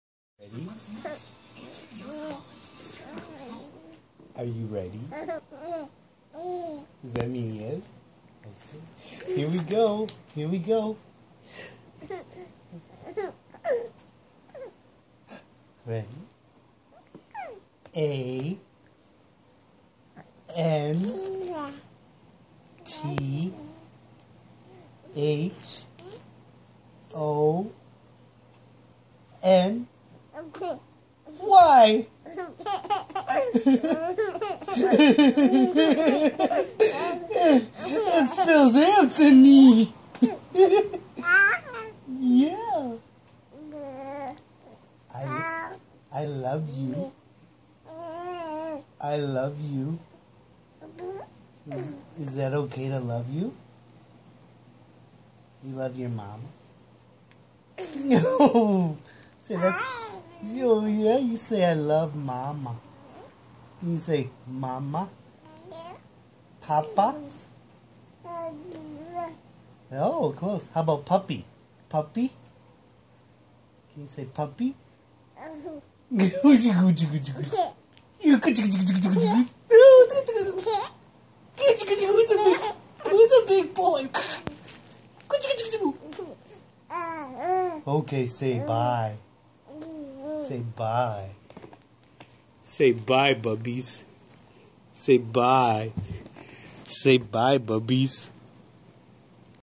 Happy laughing baby Anthony
4 month old baby Anthony. Happy as can be.